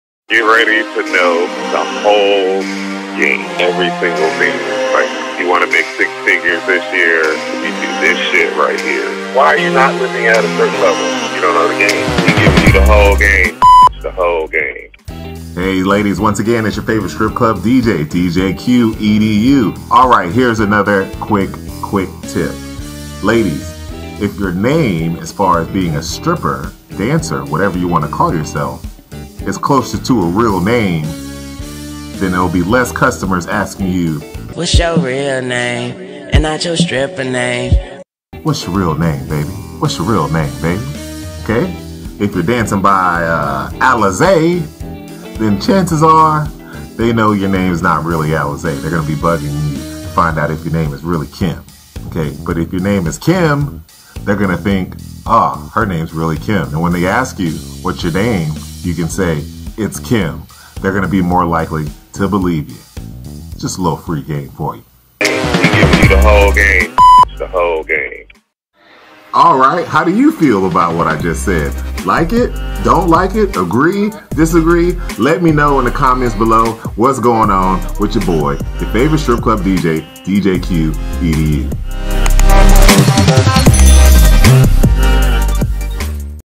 (Stripper) Game: How To Pick Your Stripper Name!!!
It's hard to know what name to call yourself in the strip. This video gives you a clue on how to pick a name and which names to stay away from and why.